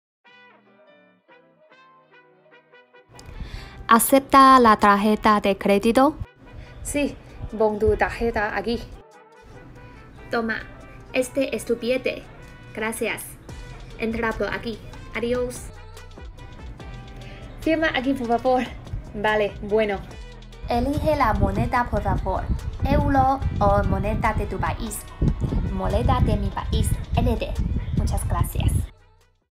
💛Práctica-23
A: ¿Acepta la tarjeta de credito?
B: Sí, pon tu tarjeta aquí.

A: Toma, este es tu billete.
B: Gracias.
A: Entra por aquí. Adios.

A: Firma aquí, por favor.
B: Vale. Bueno.

A: Elige la moneda, por favor. ¿Euro o moneda de tu país?
B: Moneda de mi país. NT, muchas gracias.